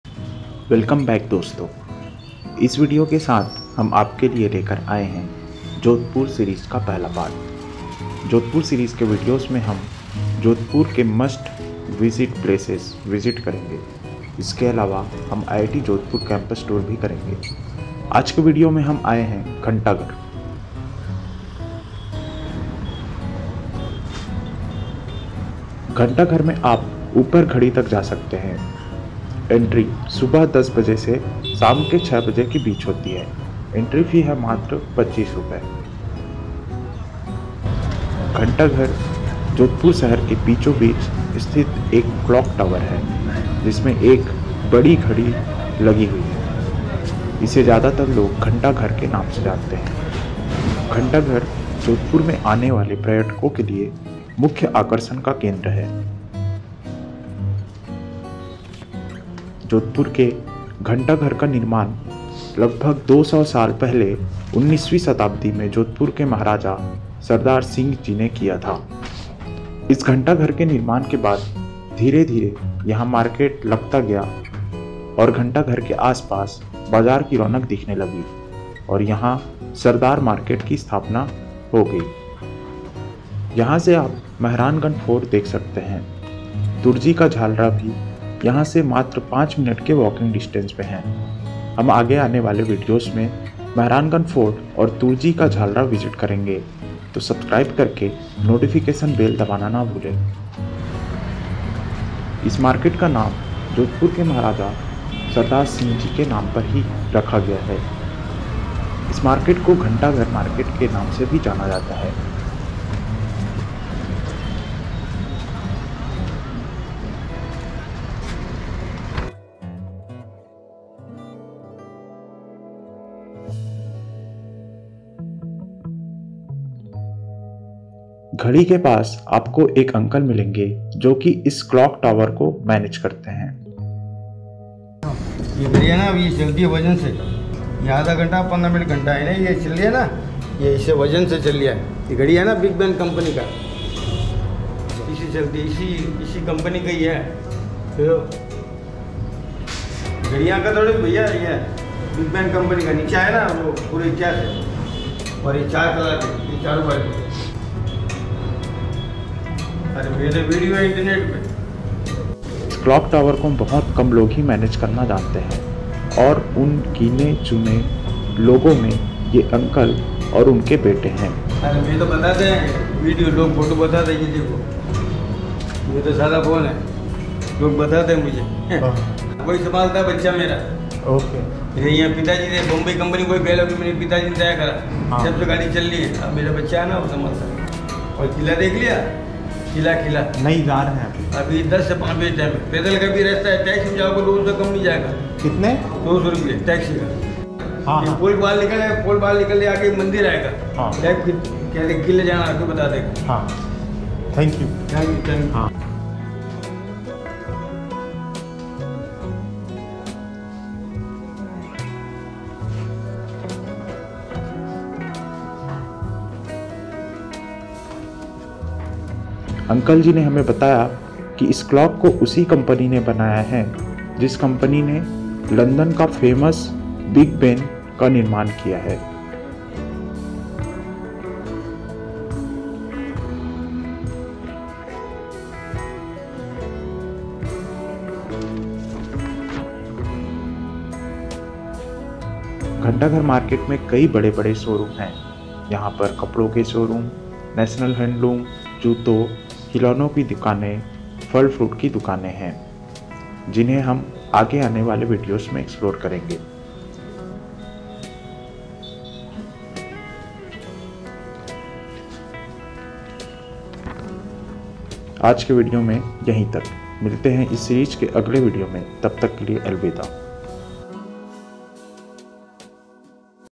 Clock Tower ( घंटाघर ) | JODHPUR DIARY | Full Detail
Ghanta Ghar or the Clock Tower is a prominent landmark in Jodhpur; it was built by Maharaja Sardar Singh between 1880 and 1911. More of a landmark in the market by the same name, it is more of an architectural delight.